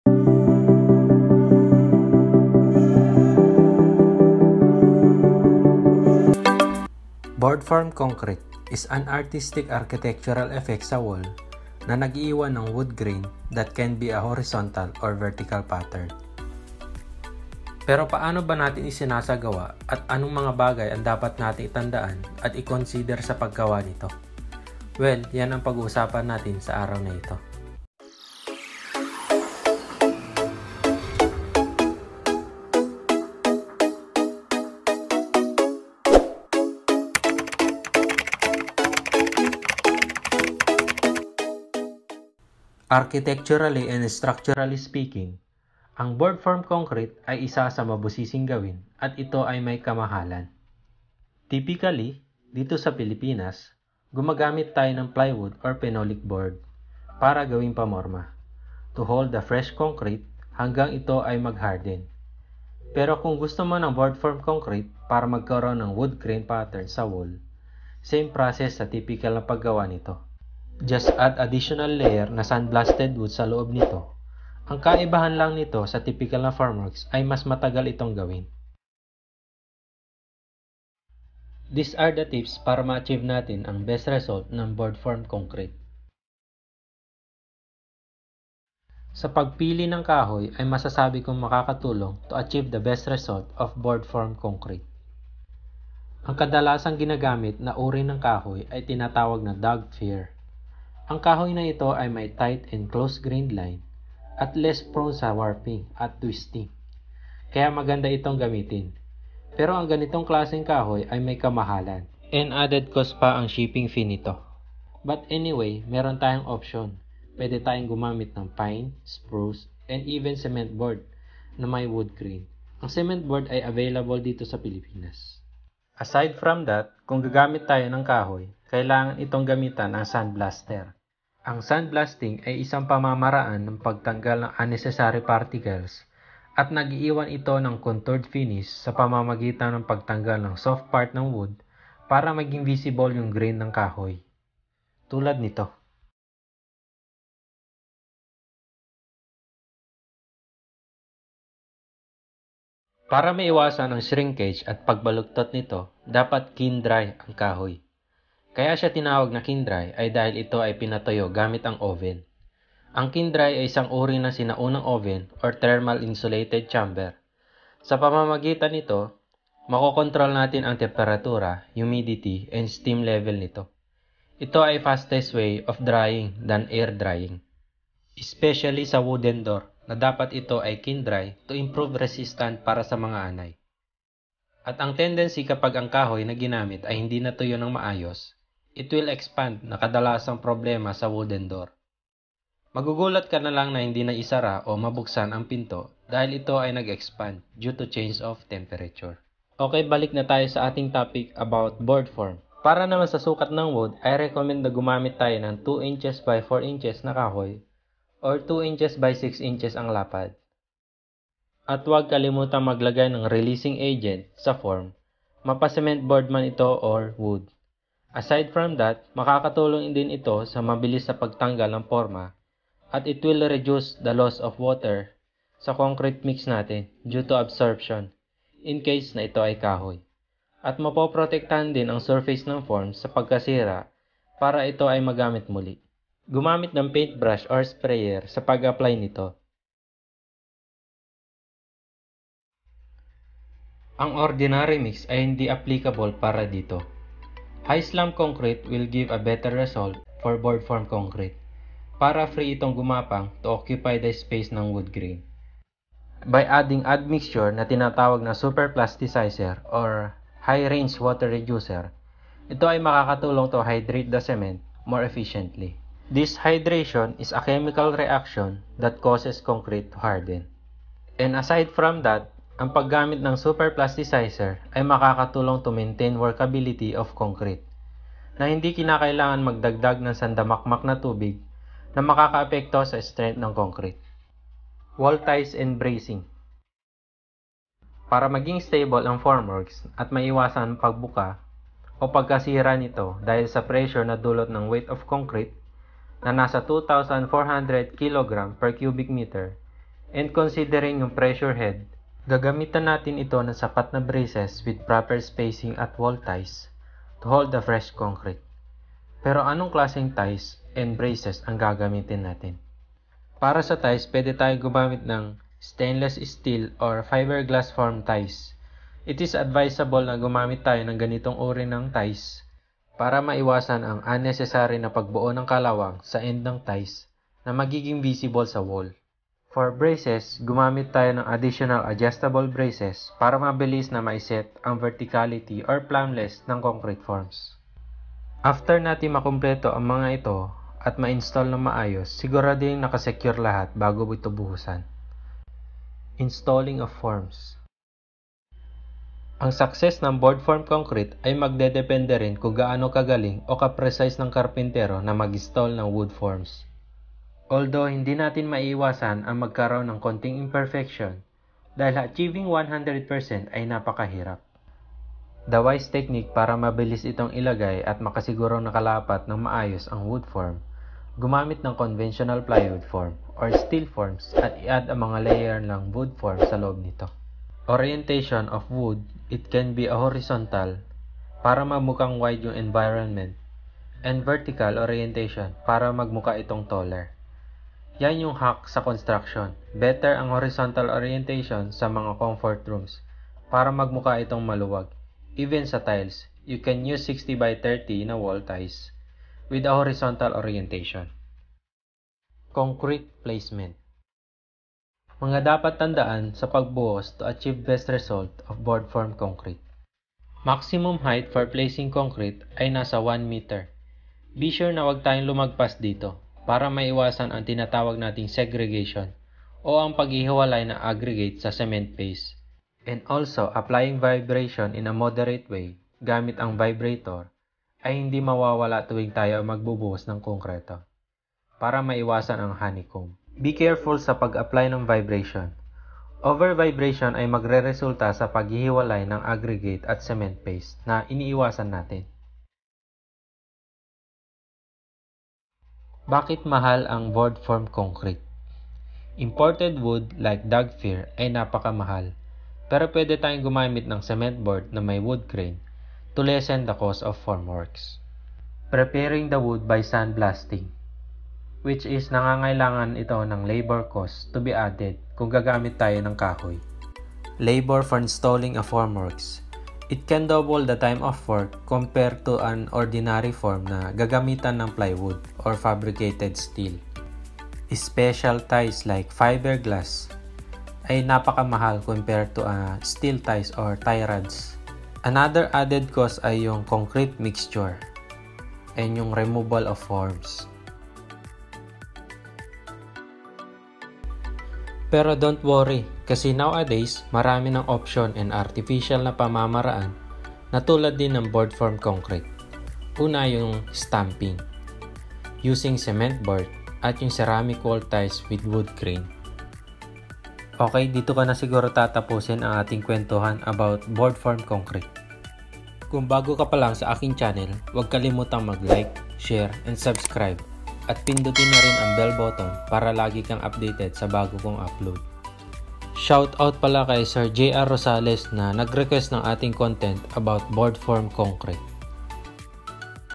BOARD-FORMED CONCRETE WALL
It is an artistic Architectural effect sa wall na nagiiwan ng wood grain sa Concrete.

Ang nilalaman ng video na ito ay patungkol sa mga gabay para makabuo na isang Board-formed concrete mula sa paghahalo ng Concrete, paglalagay ng porma at braces tamang pag apply ng vibration, at higit sa lahat ang tamang pagpili ng kahoy para gawing muldura upang makabuo ng perfect Board-formed Concrete Wall at tatalakayin din natin ang mga ibang materyales as an alternative sa kahoy.